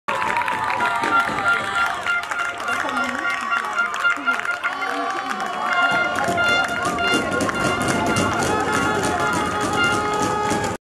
Schlusspfiff in Oberzell - Die C-Junioren sind Meister
Mit 3:1 siegen die C-Junioren in einem hart umkämpften Spiel in Oberzell, holen sich den siebten Sieg in Serie und sind als Aufsteiger Meister der Leistungsklasse. Mega, herzlichen Glückwunsch !